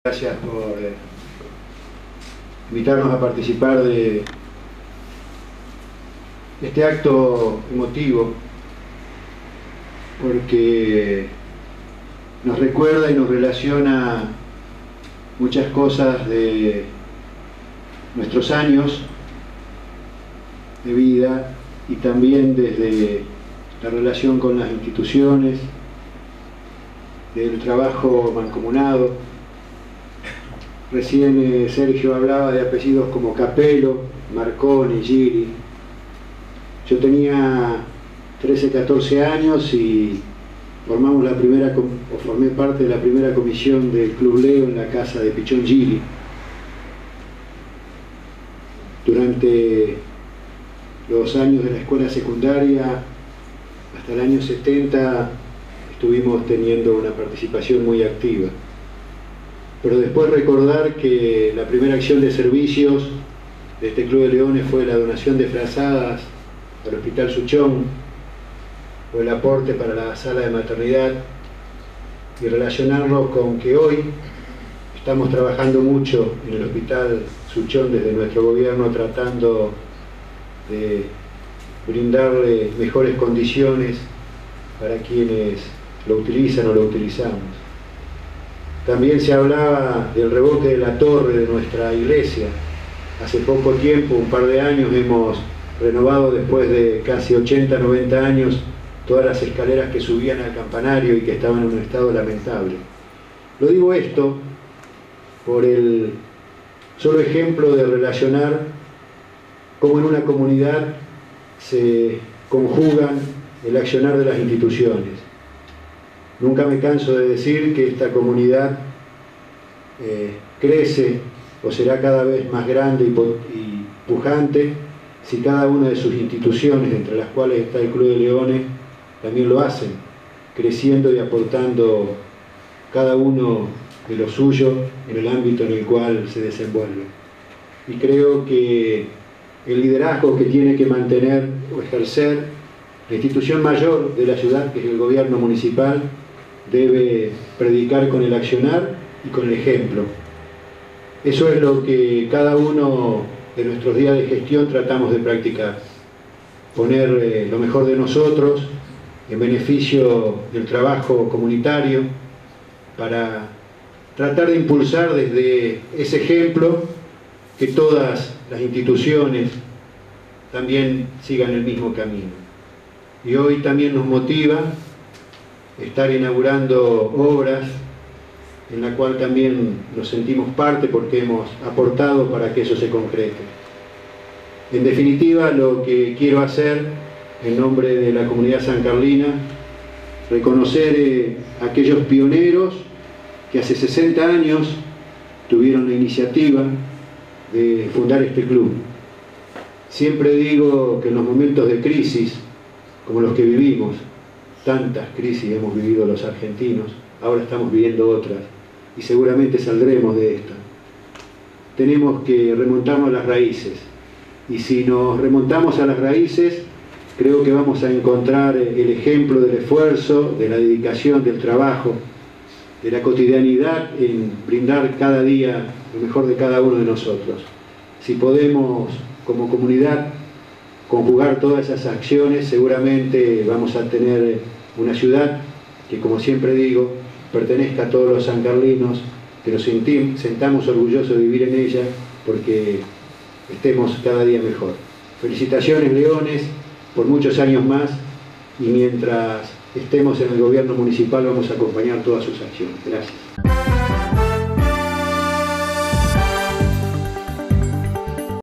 ING. JORGE PLACENZOTTI – JEFE DE GOBIERNO
ACTO POR LOS 60 AÑOS DEL CLUB DE LEONES